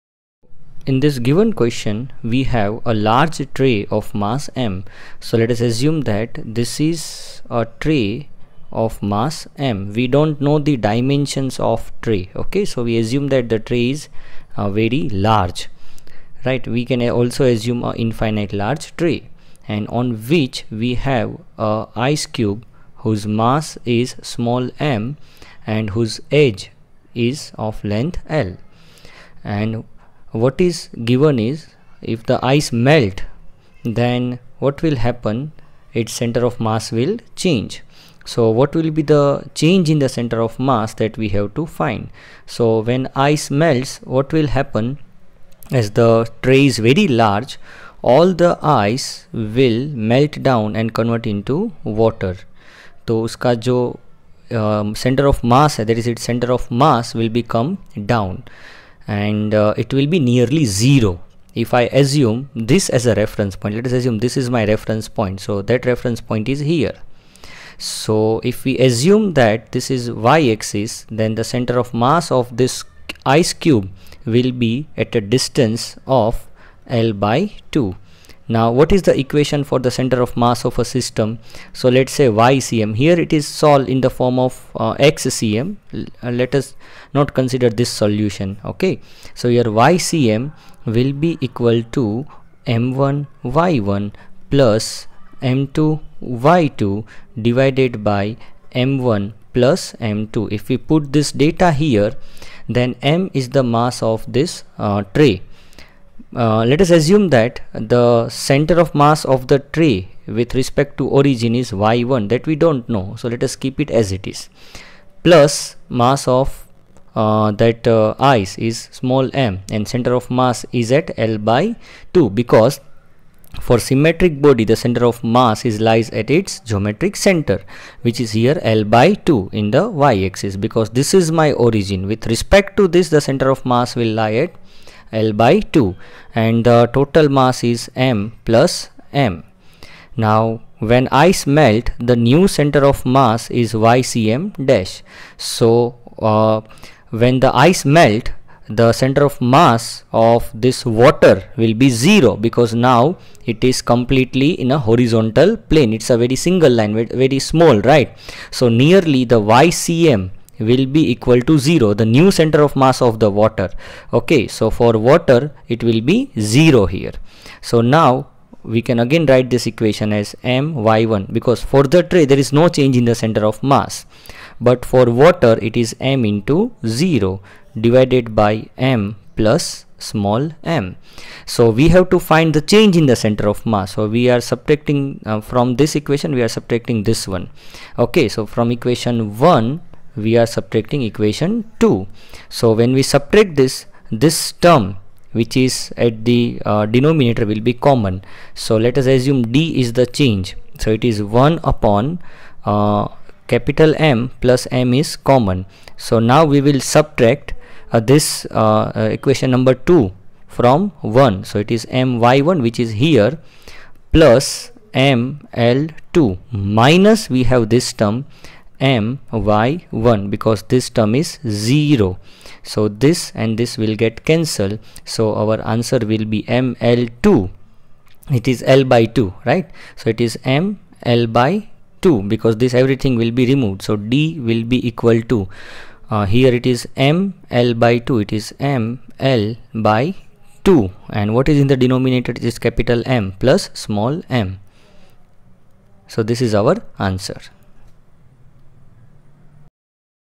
On large tray of mass M an ice cube of mass m and edge I is kept. If the ice melt completely, the
On large tray of mass M an ice cube of mass m and edge I is kept. If the ice melt completely, the centre of mass of system comes down by .